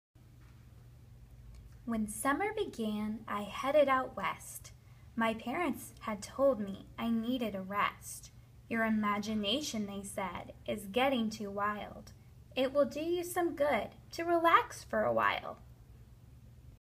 How I Spent My Summer Vacation by Mark Teague
Follow along and practice fluency with Mrs. Barnes as she reads page 1 of How I Spent My Summer Vacation